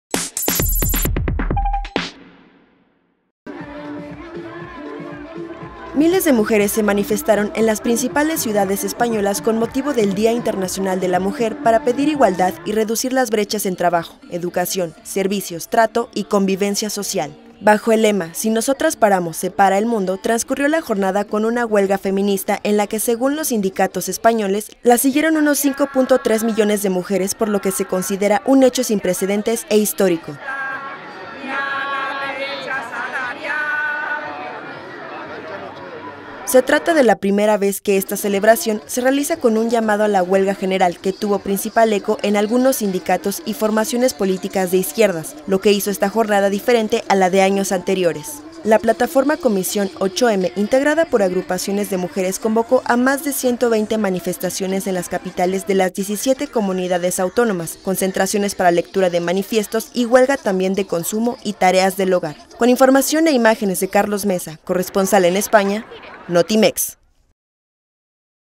Miles piden igualdad en España, en el Día de la Mujer
Miles de mujeres se manifestaron en las principales ciudades españolas con motivo del Día Internacional de la Mujer, para pedir
igualdad y reducir las brechas en trabajo, educación, servicios, trato y convivencia social